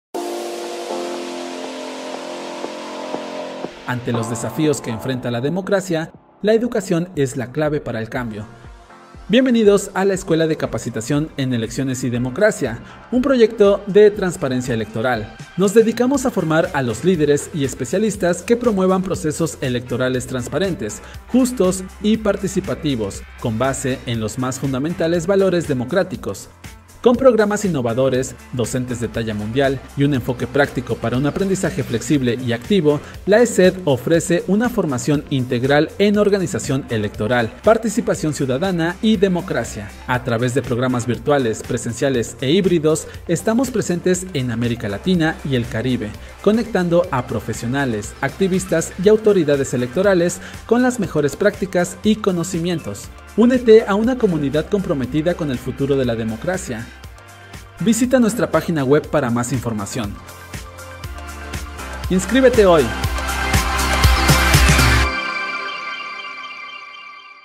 Escuela de Capacitación en Elecciones y Democracia de Transparencia Electoral
Conoce la Escuela de Capacitación en Elecciones y Democracia de Transparencia Electoral: un espacio para la formación y difusión de contenidos y prácticas que promuevan la democracia, la inclusión, la transparencia, la legalidad y el análisis basado en evidencia, con el objetivo de fortalecer la integridad de los procesos electorales en América Latina.